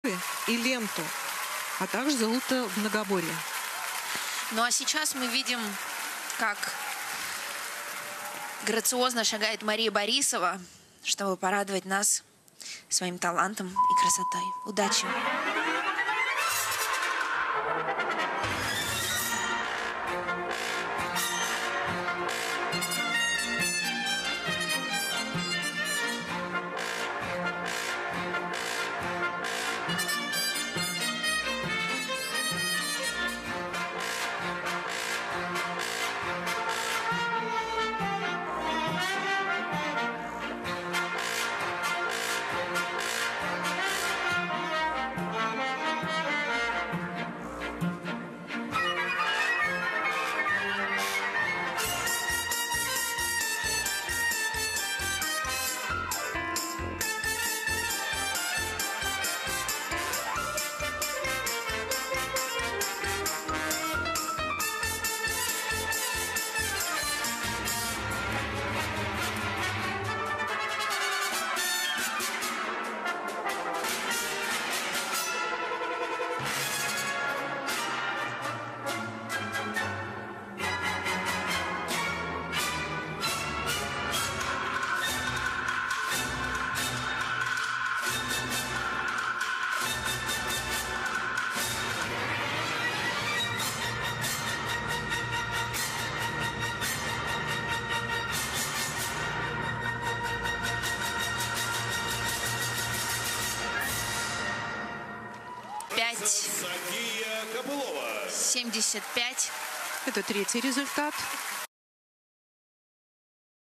Мария Борисова Обруч Кубок Сильнейших Финал
Выступление Марии Борисовой с обручем в финале Кубка Сильнейших 2023